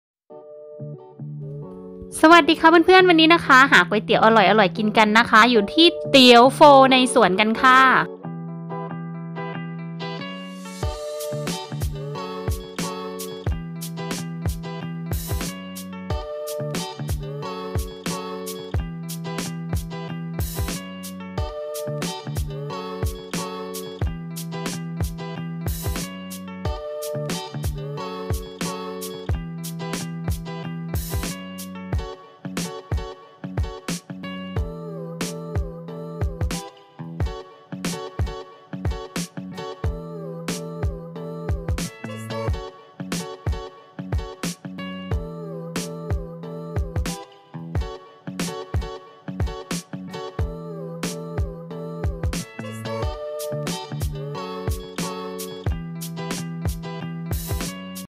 ร้านก๋วยเตี๋ยวโฟในสวน,ข้าวในสวน,กะบับโครตชีส เมืองชลบุรี
รวบรวมบรรยากาศร้าน อาหารนั่งสบาย สะอาด ปลอดภัย
เหมาะไปทั้งครอบครัวแบบสุดๆ ที่สำคัญมีหลากหลายอาหารอร่อย
ให้เลือกทานด้วย รับประกันว่าไปแล้วจะติดใจกันทั้งครอบครัว
"ก๋วยเตี๋ยว" ในสวน "เย็นตาโฟ" ในสวน "ข้าว" ในสวน
*ร้านเปิด วันอังคาร - ศุกร์*
เปิดบริการตั้งแต่ 07:30-16:00 น.
เสาร์-อาทิตย์ เปิด 08:00-16:00น. (หยุดทุกวันจันทร์)
พิกัด : ติดถ.พระยาสัจจา ตรงข้ามกับร้านข้าวต้มบางปะกง2 เยื้องๆ ศึกษาภัณฑ์ชลบุรี ร้านตั้งอยู่ซ้ายมือฝั่งไปทางอ่างศิลา ก่อนถึงไฟแดงนินจา 1กม.